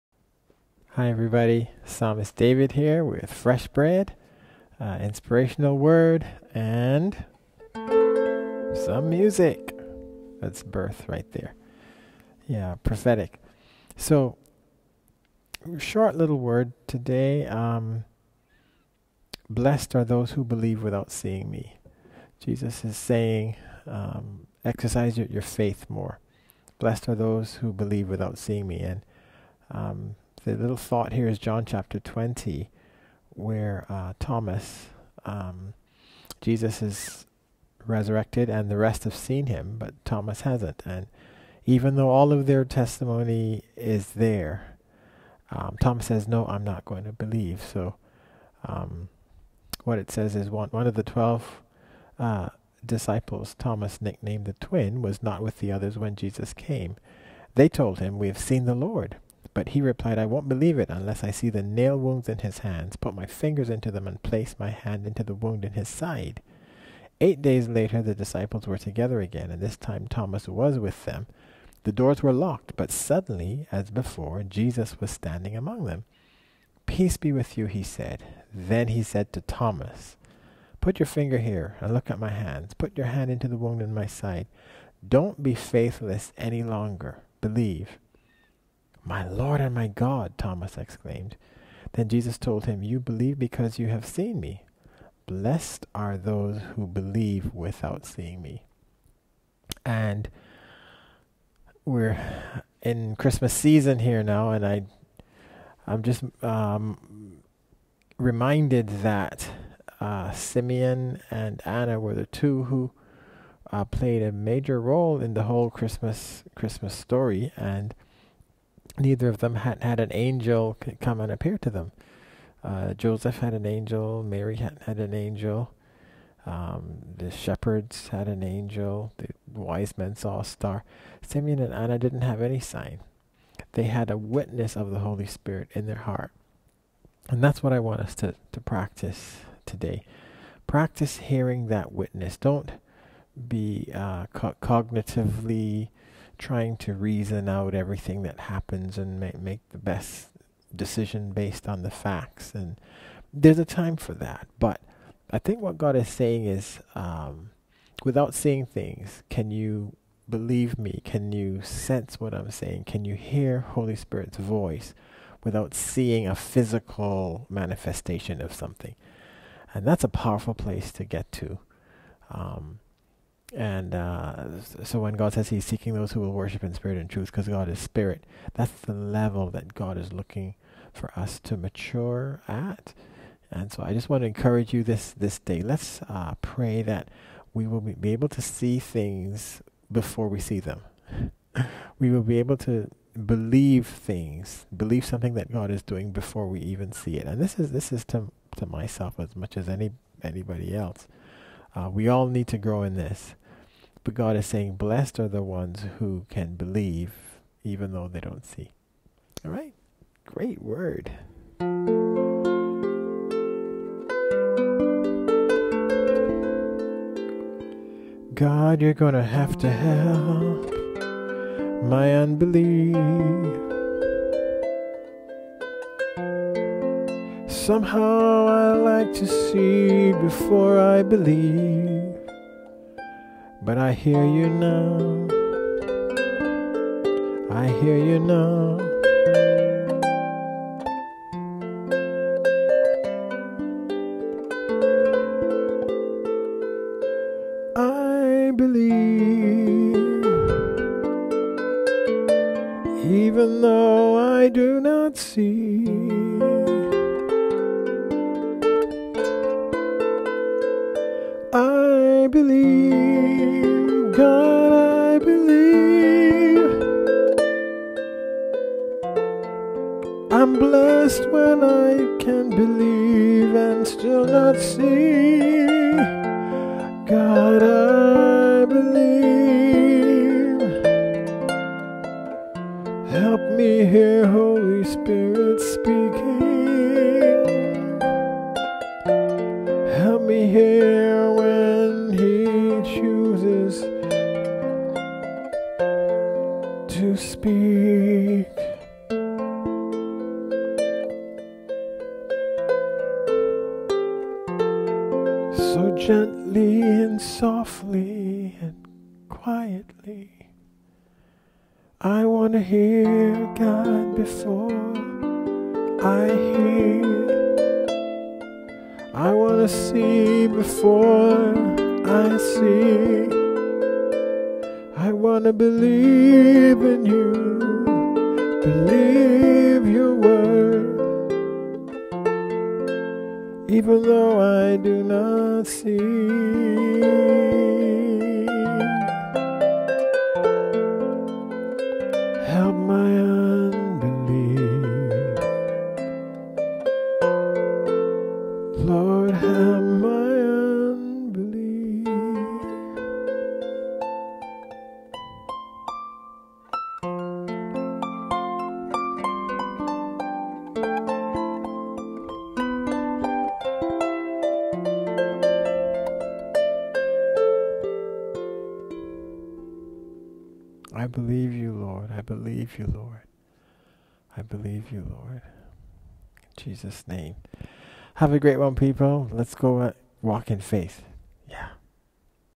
Fresh Bread 402212
"Blessed are those who believe without seeing me". Unlike Thomas, we need our faith to go ahead of us. Like Simeon and Anna, we want to hear Holy Spirit leading us.